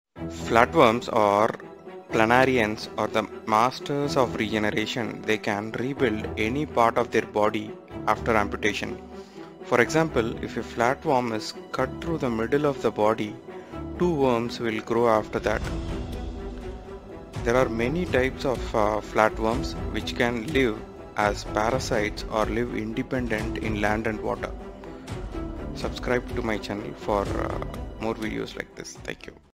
This worm can live after death
In this video we will see about flatworms.

Flatworms  or planarians are the masters of regeneration they can rebuild any part of their bodies after amputation
For example If a flatworm is cut through the middle of the body two worms will grow from that.
There are many types of flatworms which can live as parasites or live independent in land and water.

Kuriositynow facts  does not own the rights to these video clips.